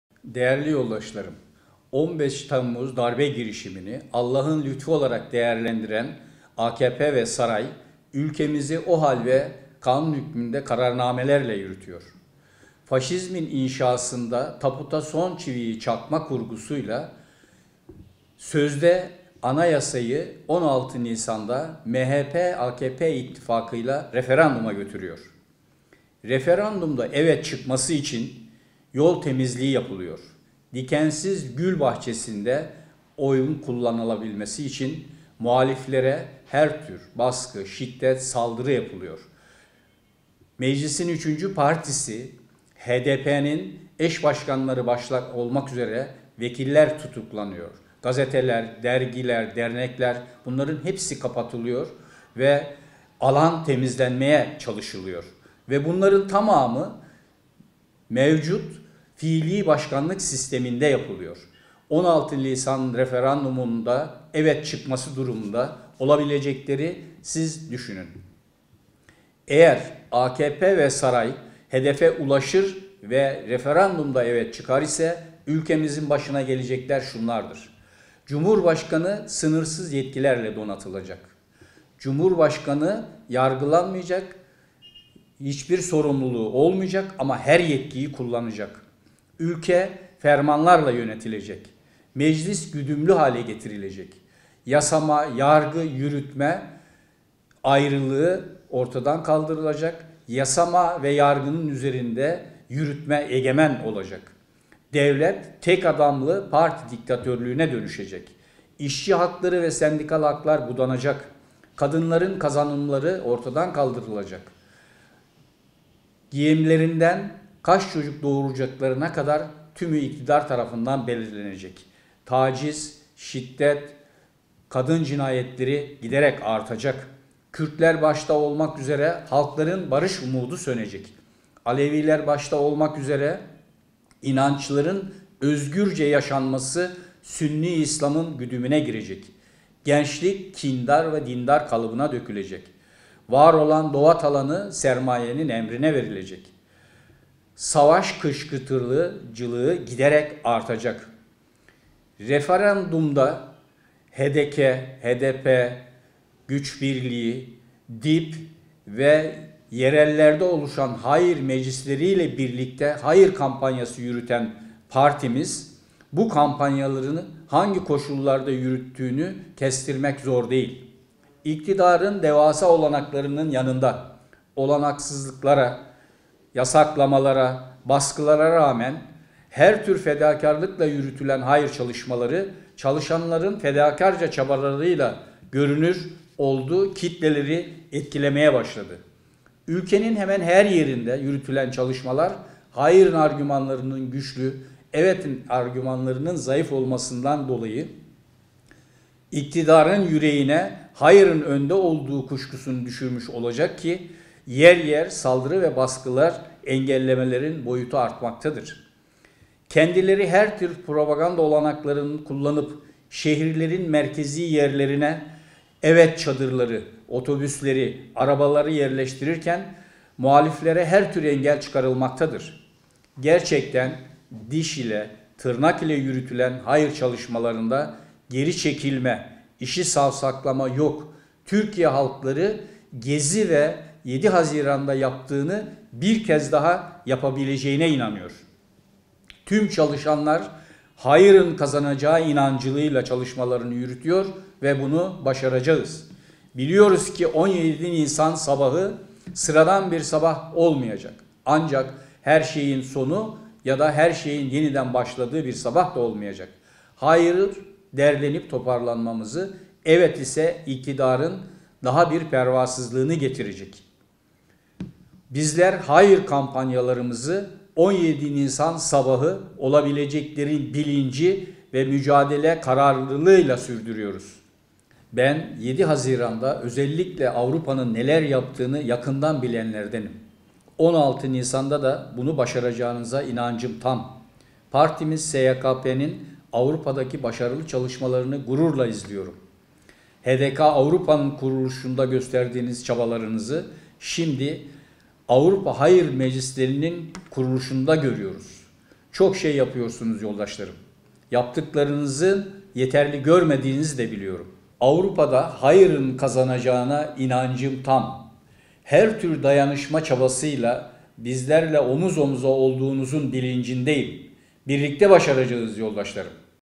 SYKP Eş Genel Başkanı Avrupa'daki Türkiyelilere seslendi / 13.03.2017
Sosyalist Yeniden Kuruluş Partisi (SYKP) Eşgenel Başkanı Ahmet Kaya, yurtdışında yaşıyana Türkiyelilere yönelik yayınladığı video mesajda, Referandumla birlikte faşizmin inşasında tabuta son çivinin çakılmak istendiğini belirterek bu gidişatı durdurmak için herkesi bütün imkanlarını sonununa kadar kullanmaya çağırdı.